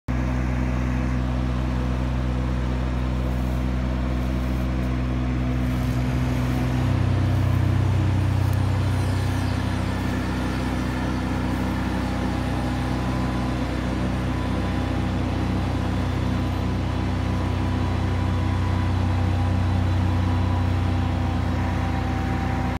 Finally getting my pond dig out!
I have waited 3 years since the major mudslide covered most of my back yard! It is amazing watching the guy work. He can even save the big rocks from the pond!